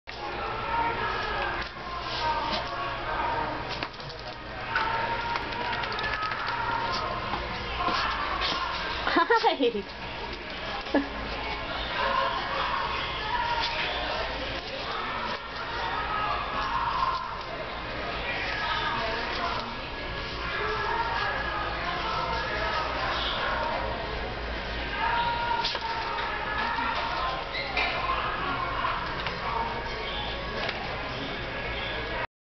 Pepper Spray poodle from cal city
Rescued from Cal city shelter so scared hardly anyone could touch her now on happy Girl!